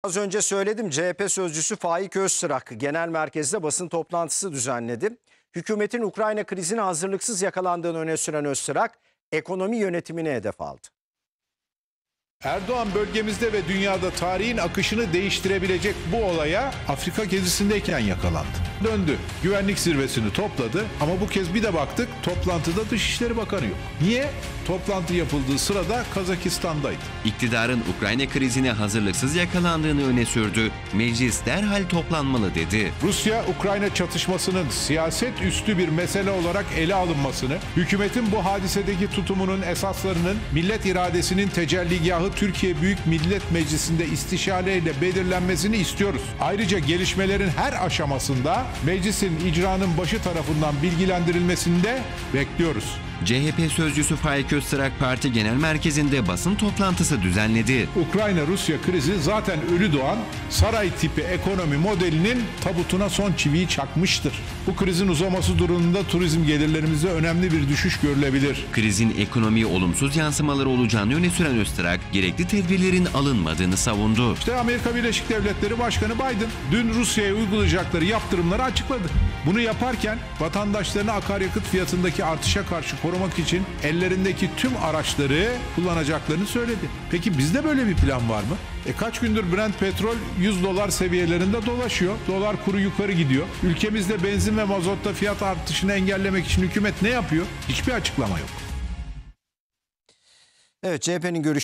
ÖZTRAK: KRİZE HAZIRLIKSIZ YAKALANDILAR
CHP Genel Başkan Yardımcısı ve Parti Sözcüsü Faik Öztrak, "Rusya'nın saldırısının, caydırıcı güç üzerine inşa edilecek diplomasiyle sonlandırılmasından yanayız. Montrö Boğazlar Sözleşmesi'ne ve uluslararası hukuka sıkı sıkıya bağlı kalınması gereğinin altını bir kere daha özenle çiziyoruz" dedi.